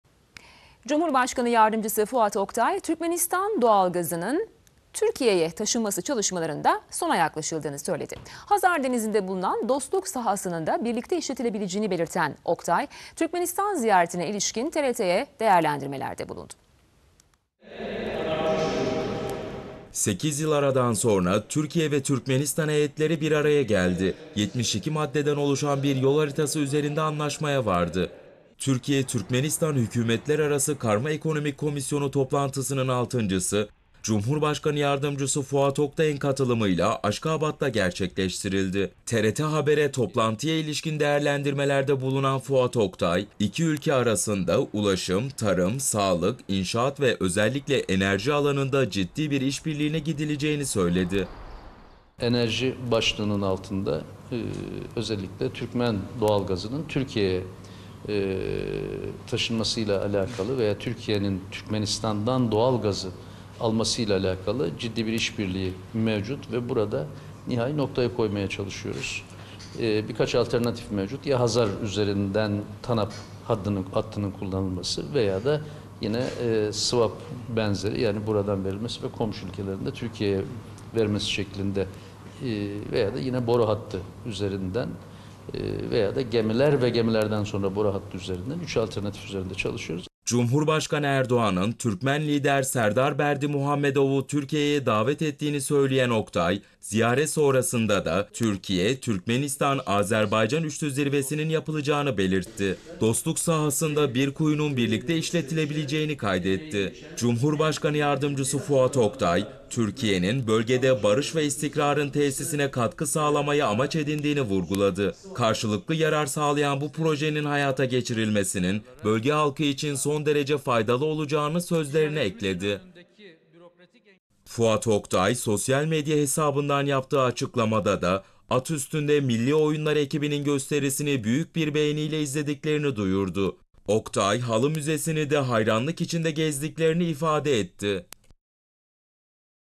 Türkmen Doğal Gazının Türkiye’ye Taşınmasında Sona Yaklaşıldı
Cumhurbaşkanı Yardımcısı Fuat OKTAY Türkmenistan Doğal Gazının Türkiye’ye Taşınması Çalışmalarında Sona Yaklaşıldığını Söyledi.
Hazar Denizi’nin Dostluk Sahasının da Birlikte İşletebileceğini Söyleyen Fuat OKTAY Türkmenistan Ziyaretine İlişkin TRT’ye Değerlenmelerde Bulundu.